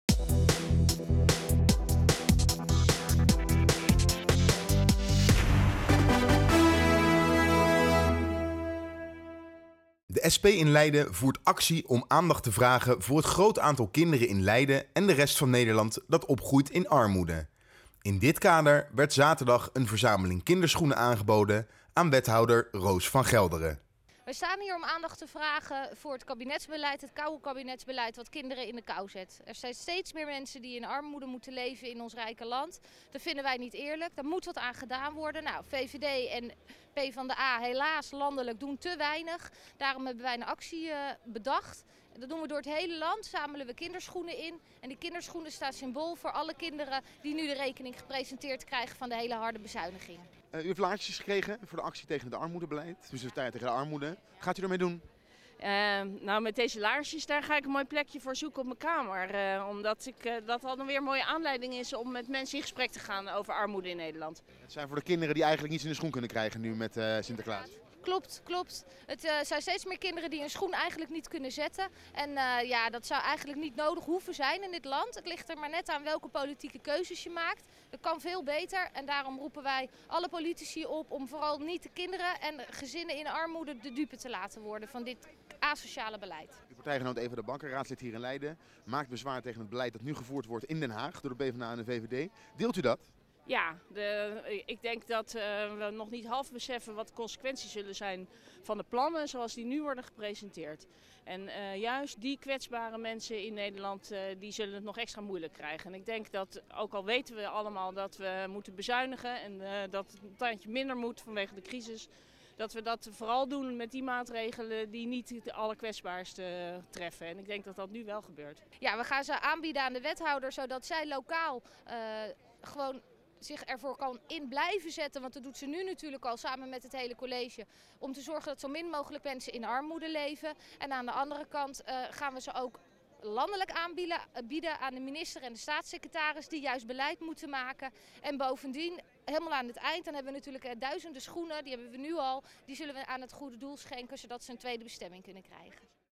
Leiden inBeeld: SP actie tegen armoede onder kinderen
De SP afdeling Leiden voert actie om aandacht te vragen voor het grote aantal kinderen in Leiden en de rest van Nederland dat opgroeit in armoede. De SP wil een landelijk plan van aanpak om ervoor te zorgen dat in Nederland niet langer kinderen in armoede opgroeien. Een verzameling kinderschoenen werd daarom aangeboden aan SP-wethouder Roos van Gelderen. (Jeugd, Zorg en Welzijn).

Uit onderzoek van de SP Leiden blijkt dat veel kinderen in armoede opgroeien. Thuis is te weinig geld voor een gezonde maaltijd, schoolreisjes, nieuwe kleren of een verjaardagscadeautje voor vriendjes. Kinderen die deze dagen hun schoen zetten treffen die steeds vaker leeg aan, de volgende ochtend.